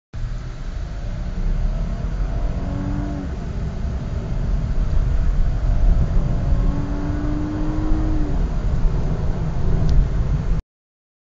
Lexus Is 300 0-100 in 7 seconds
My Lexus IS 300 2007 accelerating from 0-100 in 7 seconds.For those of you that have been commenting it did 0-100 in 10 sec's,well i had 5 friends in the car with me,and didnt change the engine oil.And this Lexus IS-300 is Middle east specs!